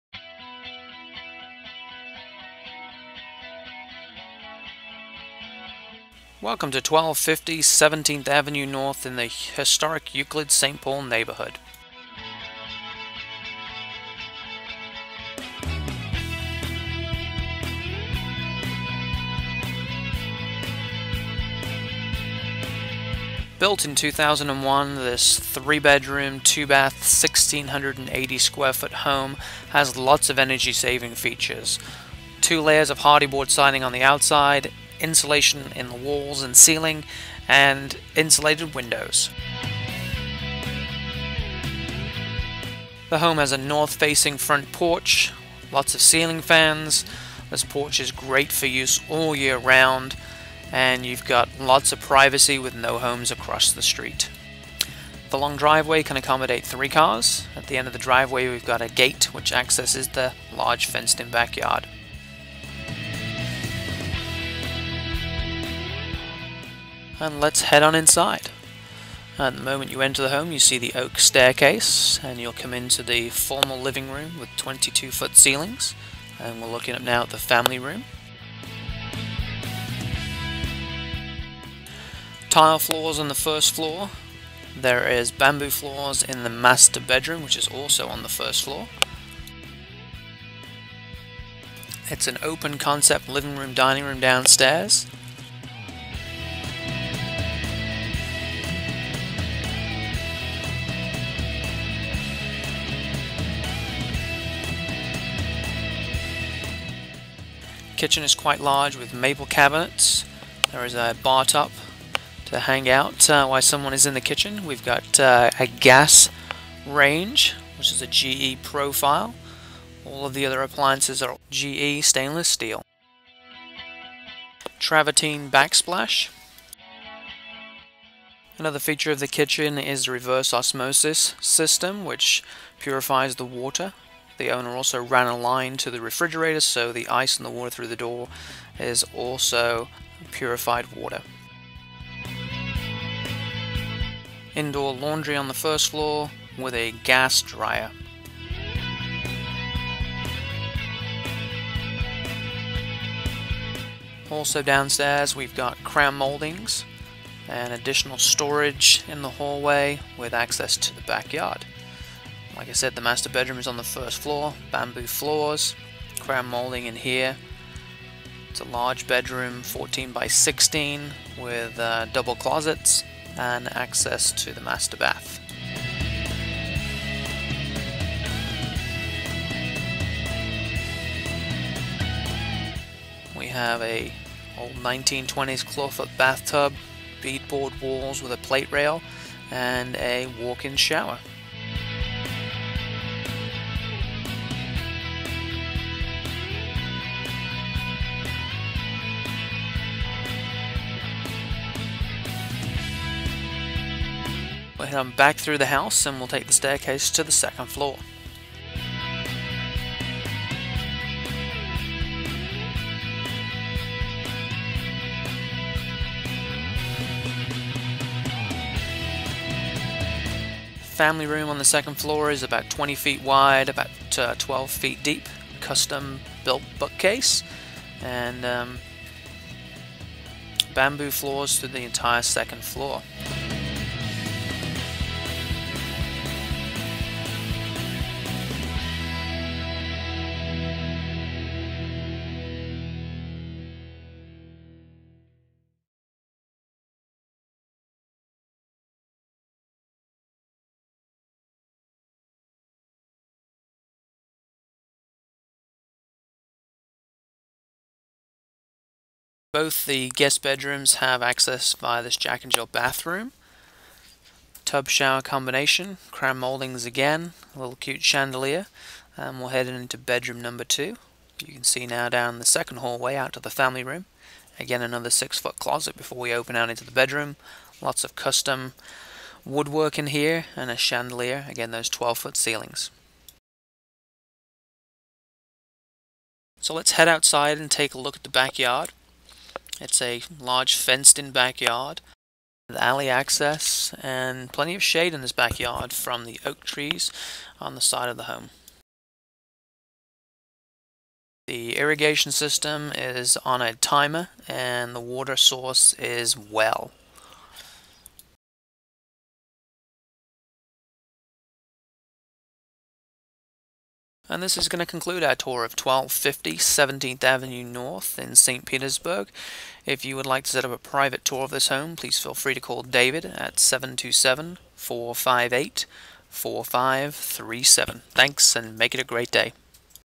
1250 17th Ave. North St. Pete FL 33704, David Price, Home for sale, price group
David Price with the Price Group presents 1250 17th Ave. North St. Pete FL 33704. Located in the Historic St. Paul/Euclid neighborhood! The first thing you'll notice about this 3 bedroom, 2 bathroom home is it's beautiful architecture and the southern style wrap around porch that overlooks a wide open field. You get a feeling of privacy from the lush landscape and the fact that there is another open lot to the side of the home. Coming into this home you'll enjoy 22' ceilings with exposed wood beams, an open kitchen, dining room and great room plan with a loft family room. The kitchen offers loads of counter space and cabinets with a GE stainless steel appliance package. The 1st floor master bedroom is spacious featuring bamboo flooring, crown molding, double closets and access to the master bath with a claw foot bath tub and separate shower, bead board walls with picture rail. Head up the beautiful red oak staircase into the loft family room that features 13' ceilings, built in bookcase, hall closet/office, 2 more bedrooms and a Jack and Jill bathroom. Both bedrooms have 13' ceilings, bamboo floors and large closets. Built in 2001, the home has received many upgrades, some making it energy efficient including a digital thermostat, gas stove, dryer and water heater as well as insulated windows. Large, fenced in back yard shaded by oak trees, lush landscaping, sprinkler system with a well pump and alley access. Other features include crown moldings, inside laundry and plenty of storage including an oversized storage shed with electric. Low homeowner's insurance $1,306/yr, NO FLOOD. Termite warranty. Easy access to I-275, Crescent Lake Park and all the downtown activates. Owner is a licensed Realtor.